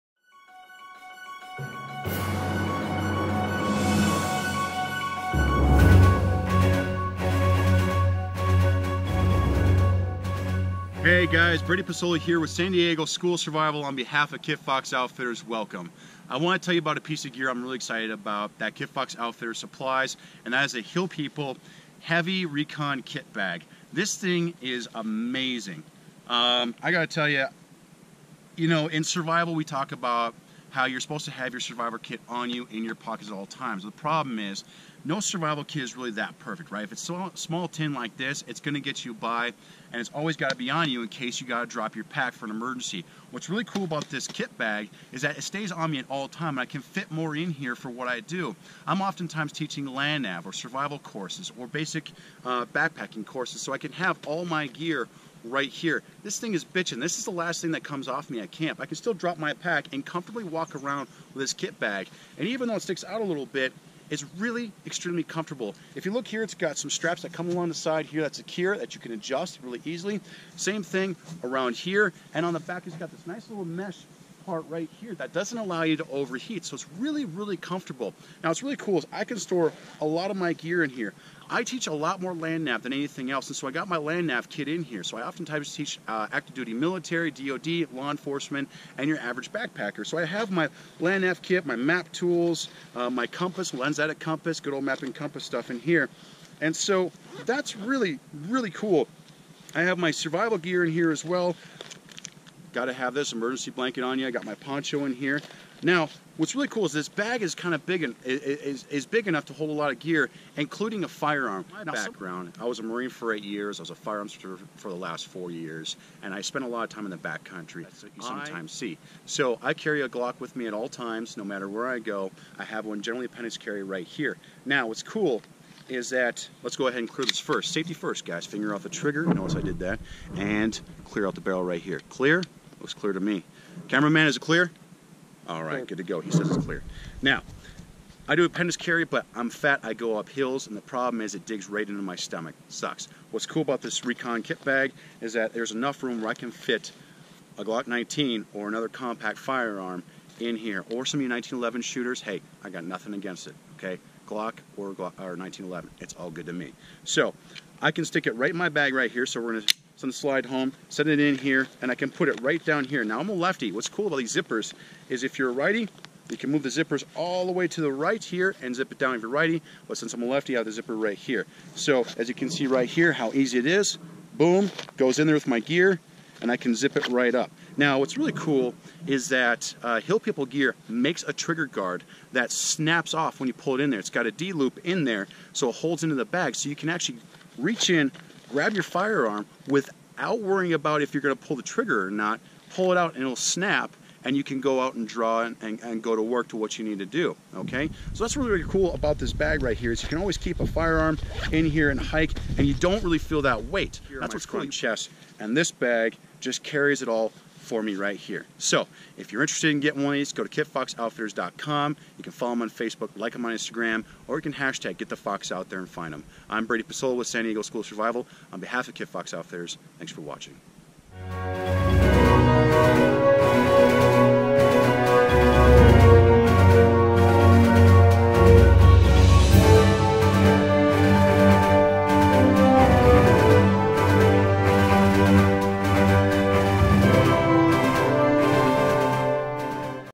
Kit Fox Outfitters, Hill People Gear’s Heavy Recon Kit Bag
A great little review with our friend Brady Pesola from San Diego School Of Survival on his new Heavy Recon Kit Bag from Hill People Gear.